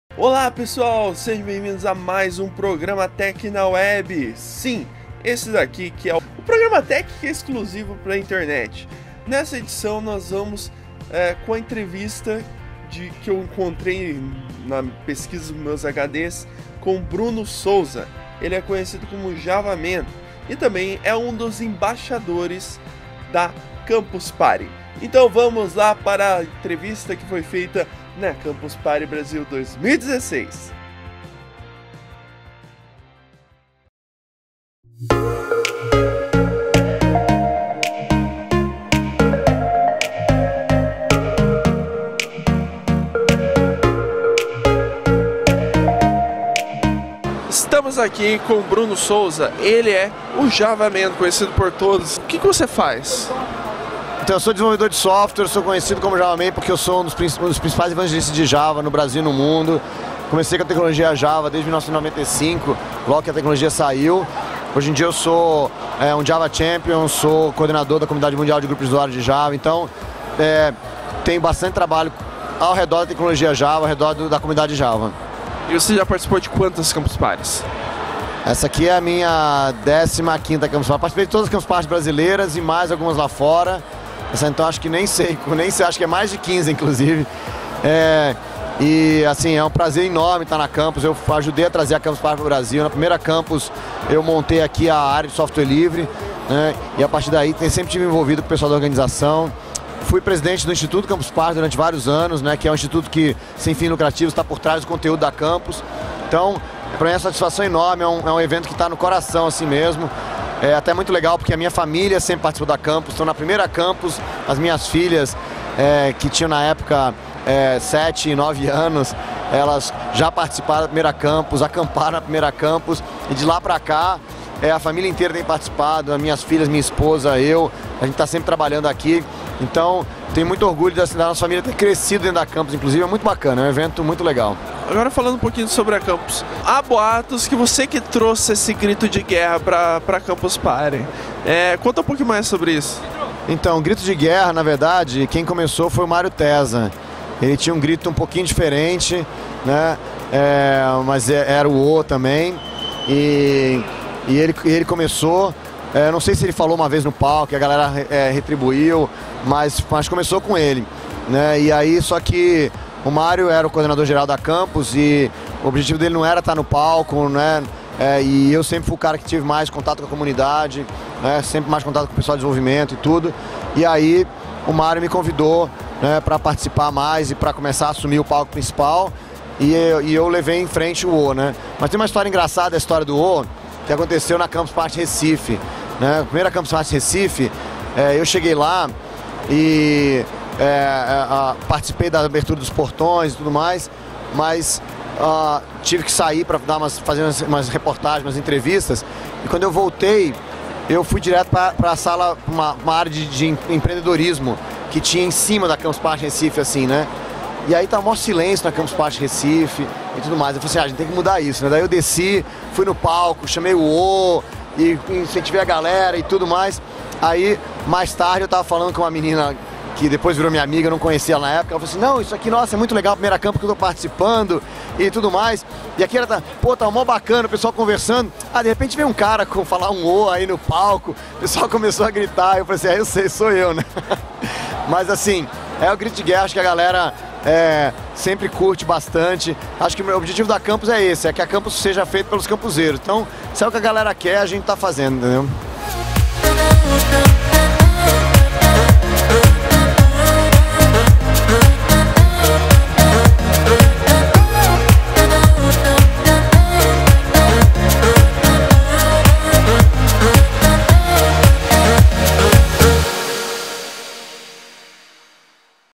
Como surgiu o Ôooooooooo!? - Entrevista com Bruno Souza (Javaman) - Programa Tech na Web
No Programa Tech na Web de hoje, a entrevista com o embaixador da Campus Party Brasil e também conhecido como Javaman: Bruno Souza, ele falou um pouco sobre ele e também sobre como surgiu o grito de guerra da Campus Party!

Corre aí assistir!